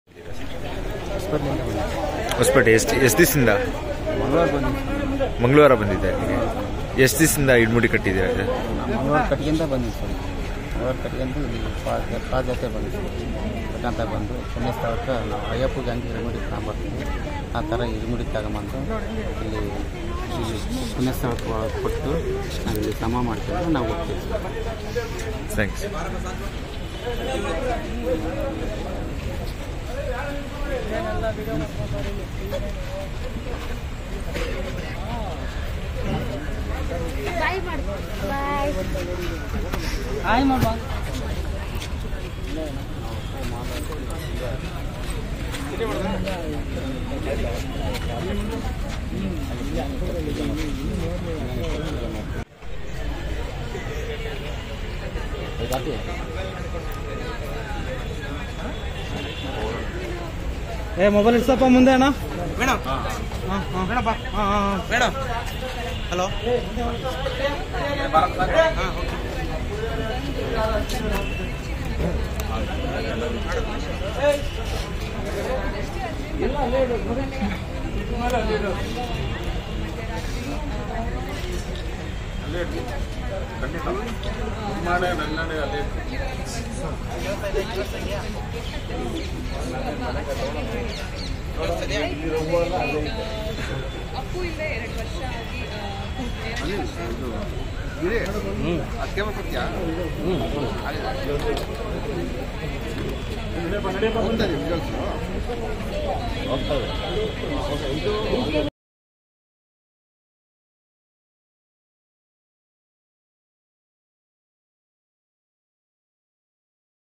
ಅಪ್ಪು ಸಮಾಧಿ ಮುಂದೆ ಮಾಲೆ ಬಿಚ್ಚಿಟ್ಟ ಸ್ವಾಮಿಗಳು | Appu maale | Puneeth Rajkumar Birthday | SStv
bangalore
Karnataka fans
Production by:-
SStv
-: SStv PRODUCTIONS :-